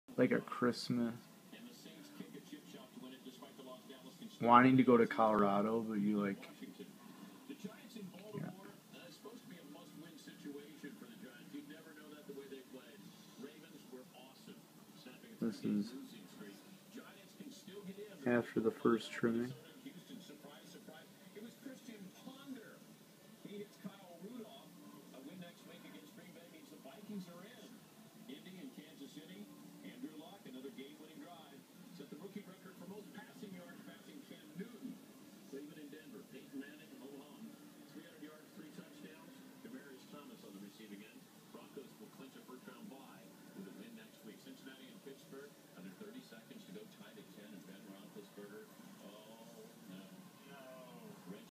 Vivarium Dart Frog Tank Update
Full tank trim to promote wider growth and new batch of accent leaves.